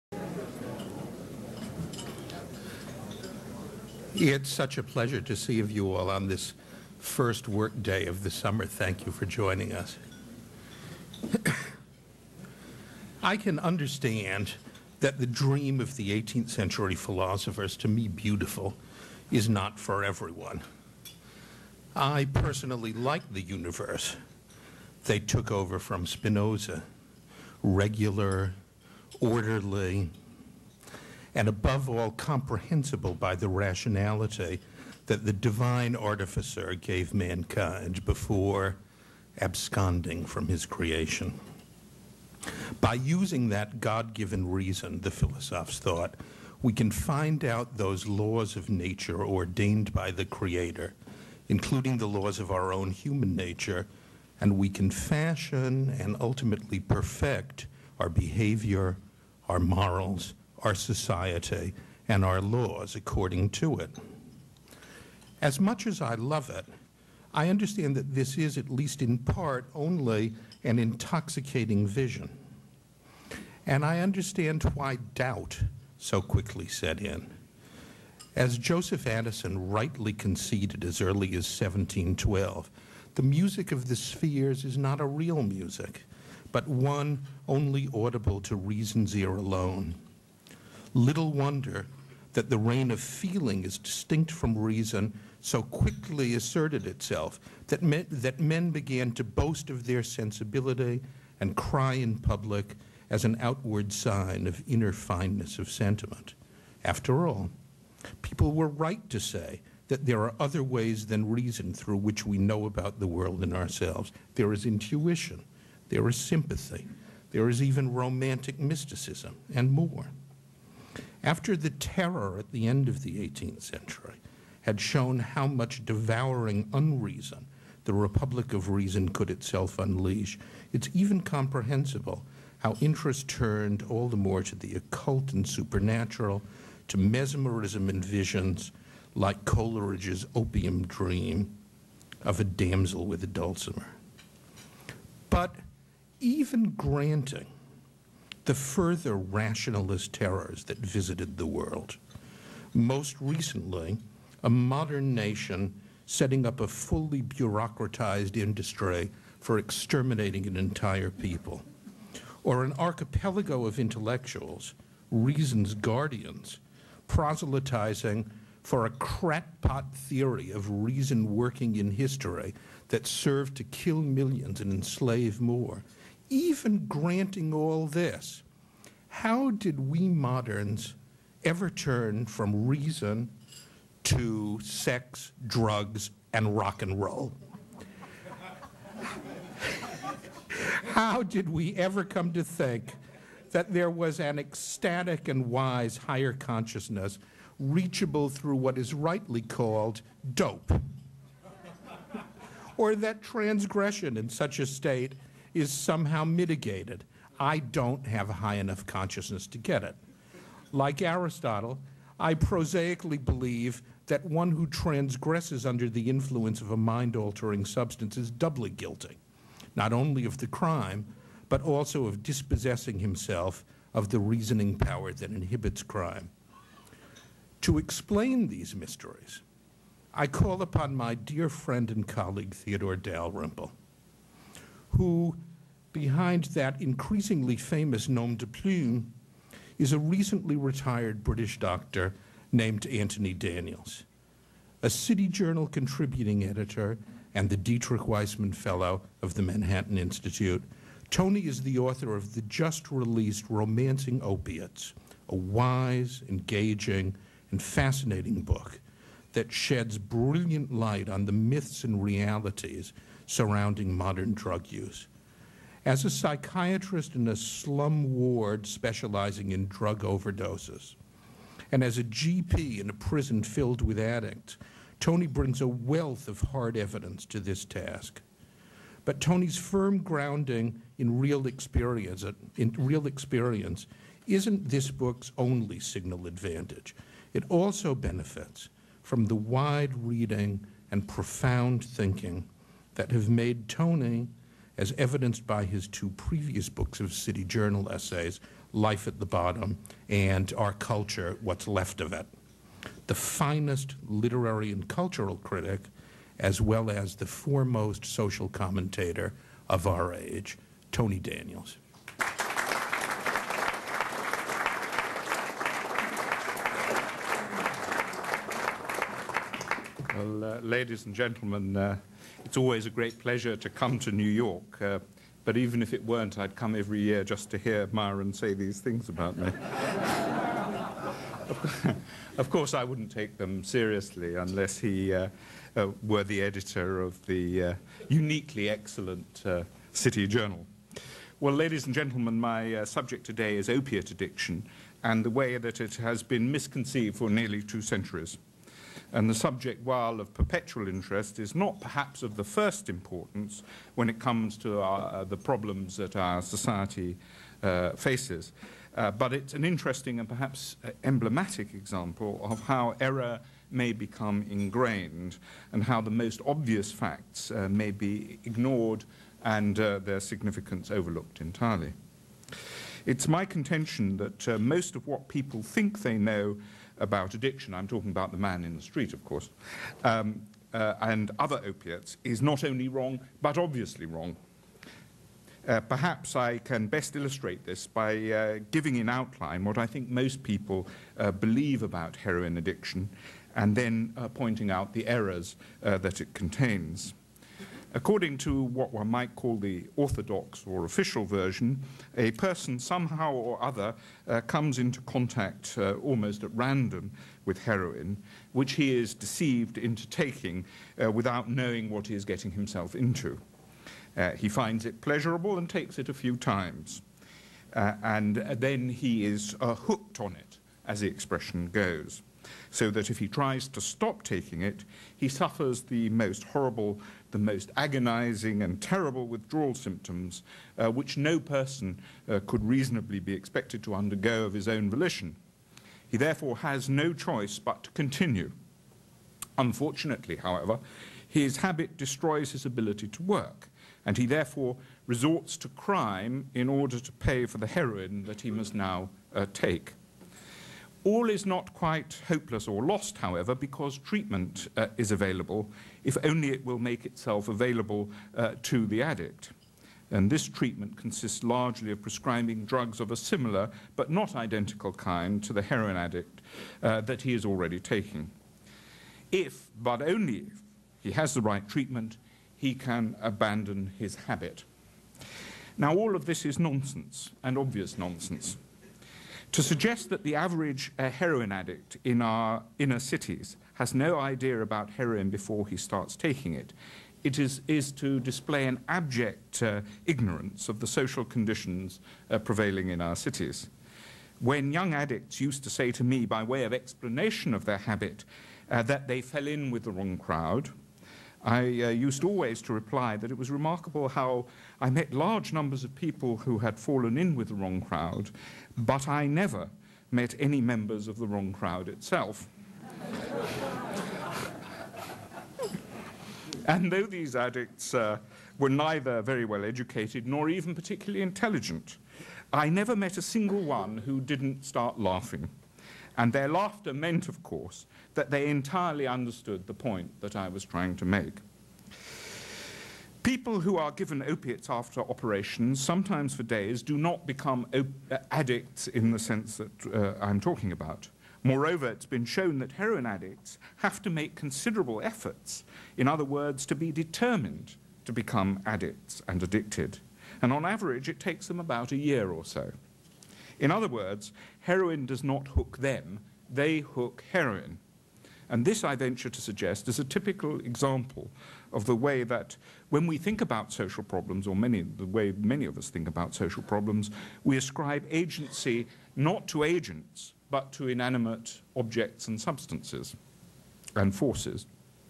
Theodore Dalrymple - Romancing Opiates - May 2006 (1 of 6)
On May 30, 2006, Theodore Dalrymple marked the release of his book "Romancing Opiates: Pharmacological Lies and the Addiction Bureaucracy" with a speech at the Harvard Club in New York. The book was published by Encounter Books, and the event was hosted by the Manhattan Institute. an unofficial website devoted to the man and his work.